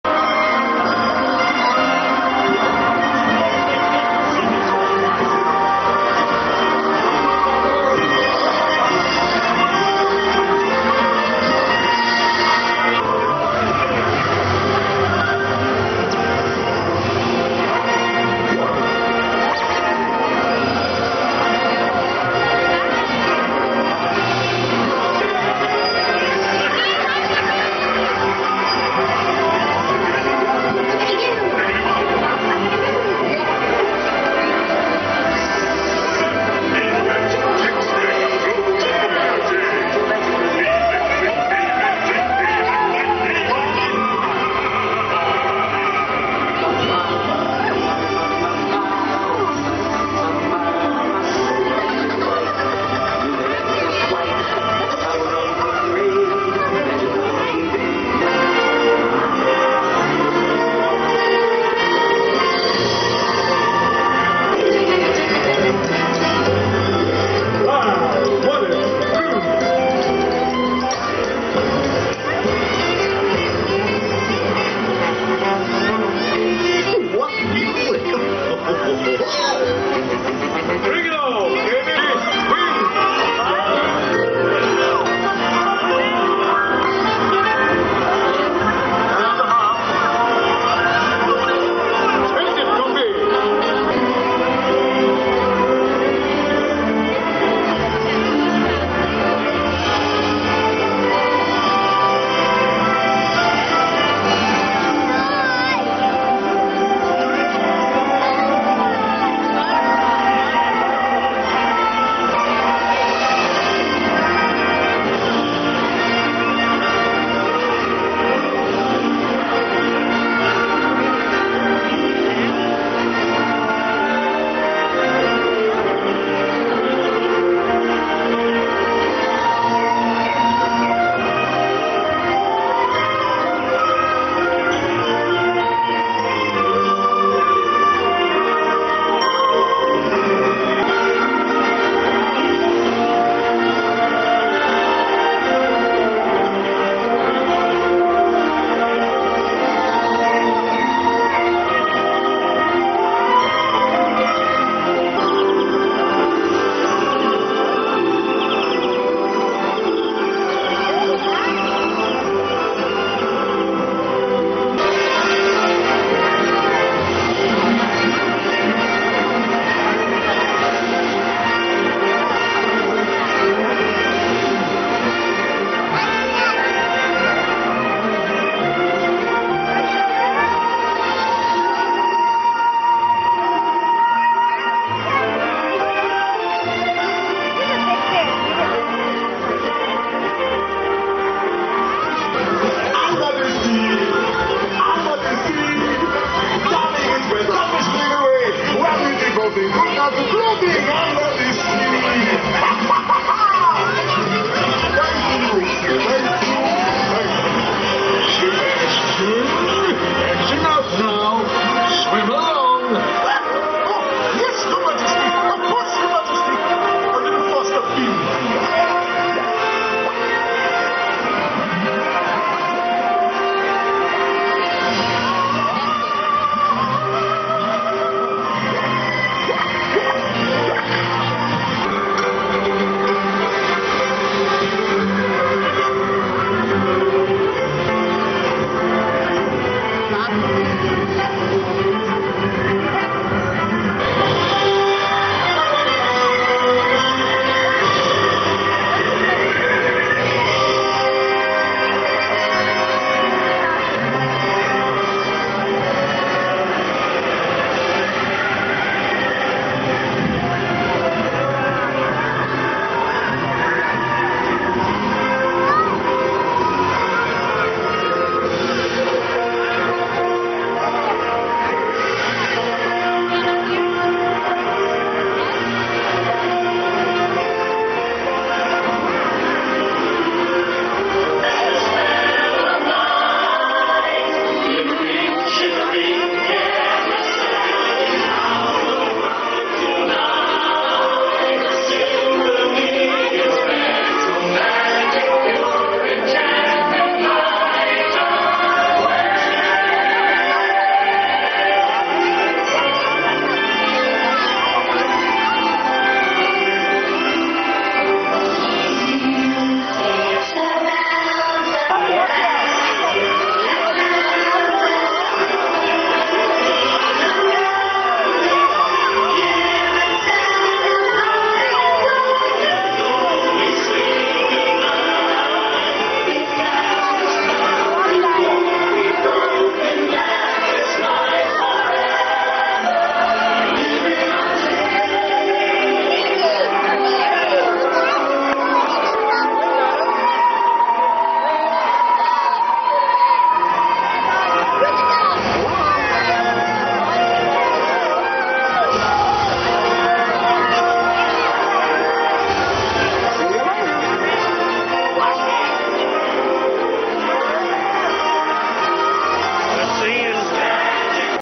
2005 - Orlando - Magic Kingdom - Night Parade - 1
This is the first of two videos made of the Night Parade in Disney's Magic Kingdom at Disney World in 2005